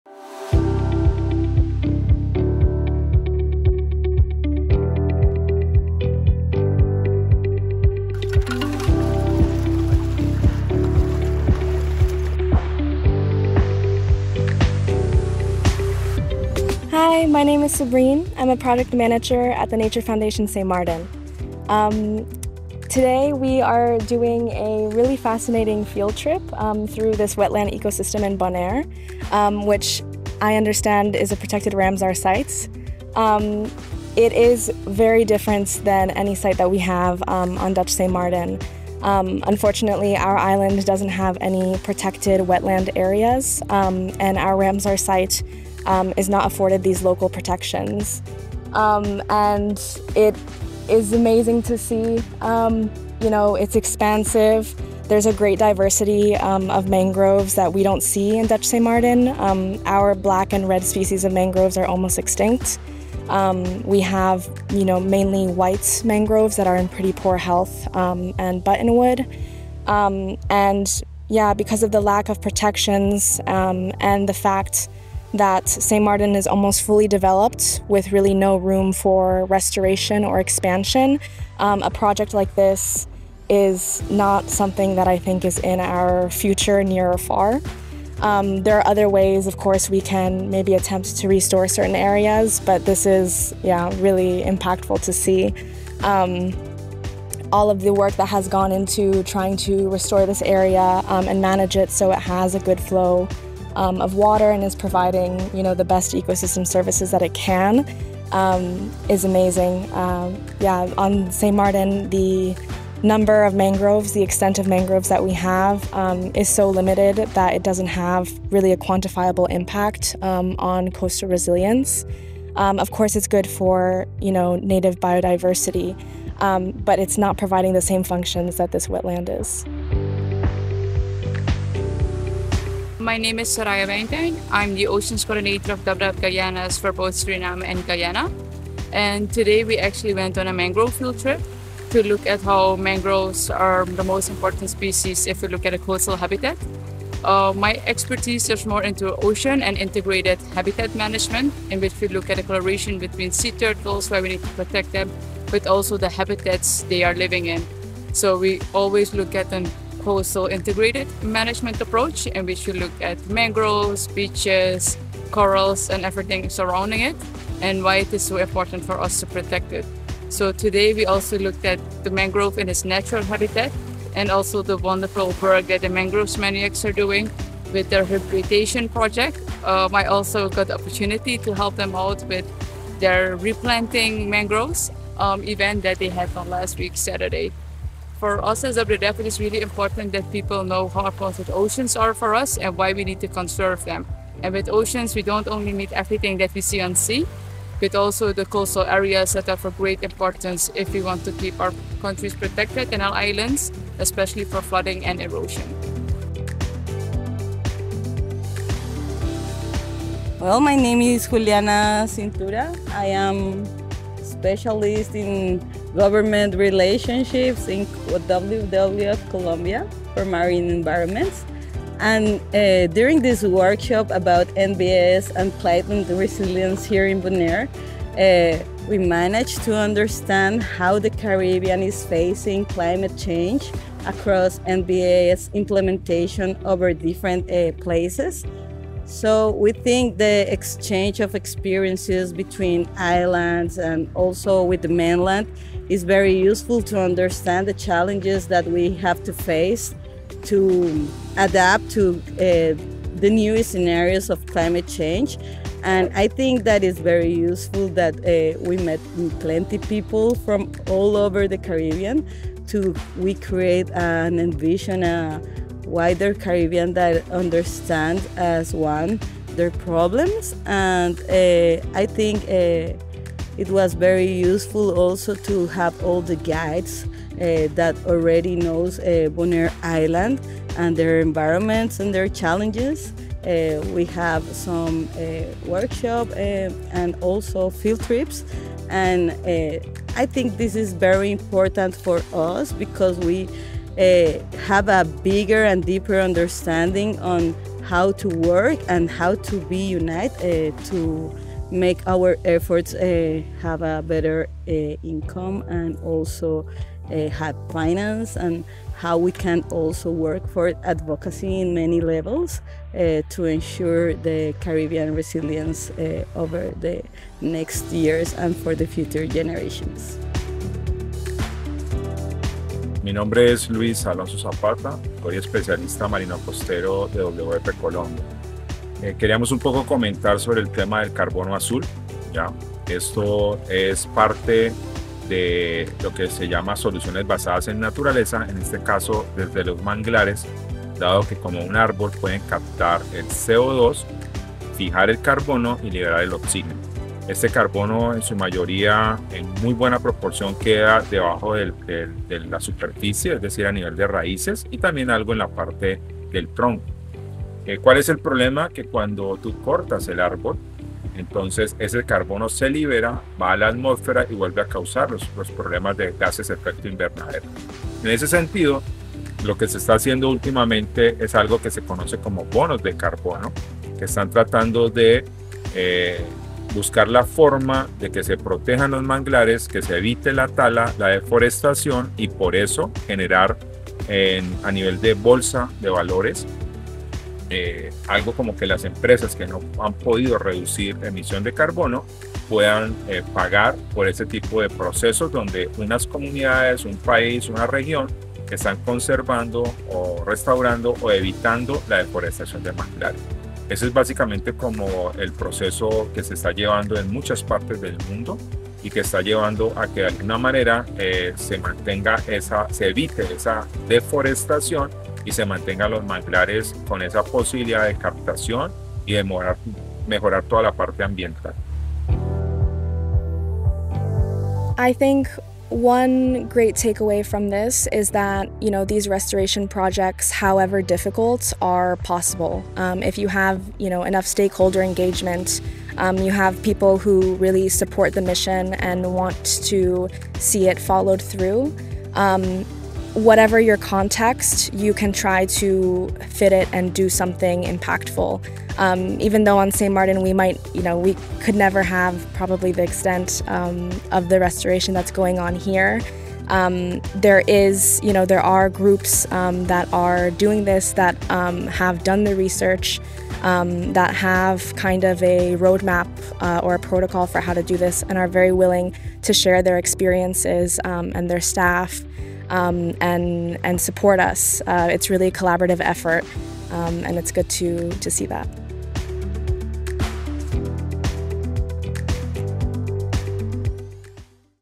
Nature Based Solutions for Climate Resilience Workshop (Bonaire) 2025
Join us for an inside look at the field work and highlights from the 2025 “Nature-Based Solutions for Climate Resilience” workshop, hosted by Mangrove Maniacs on Bonaire. Between October 13–17, conservationists, NGO staff, marine-protected area managers, scientists, and policymakers from across the Caribbean gathered to learn, collaborate, and take action to protect coastal ecosystems.

In this video, you’ll see:

On-the-ground restoration efforts among mangroves, an essential part of the island’s natural coastal defense system.

This workshop emphasizes the power of nature-based solution,  restoring and protecting healthy ecosystems to sequester carbon, improve biodiversity, safeguard coastal zones, and build resilience against climate change.